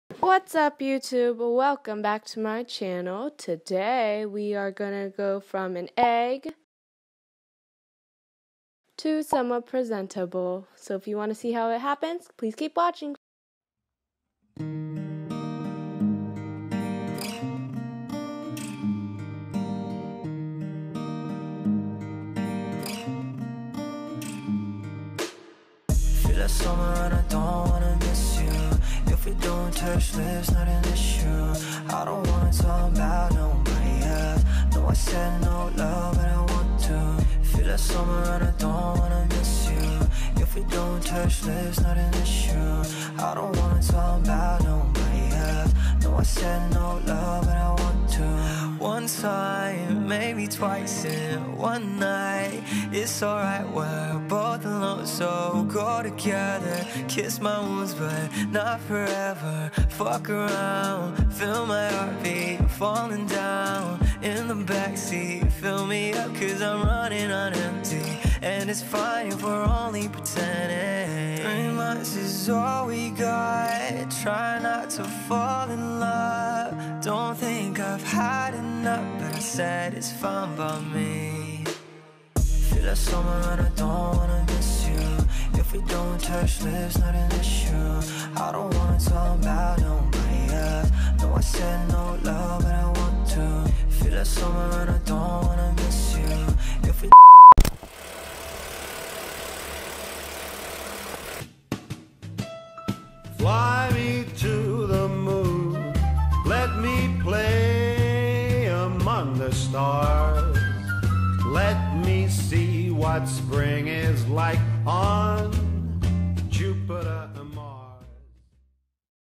Drugstore Summer Makeup // Emily Zeng
hello hello! i'm finally on summer break, so hopefully i can be much more consistent in my uploads. here's a quick easy summer grwm!

products used
- wet n wild cushion foundation in honey beige
- wet n wild coloricon blush in keep it peachy
- wet n wild highlight in precious petals
- elf mad for matte eyeshadow palette
- nyx brow mascara in brunette
- mario badescu rose water spray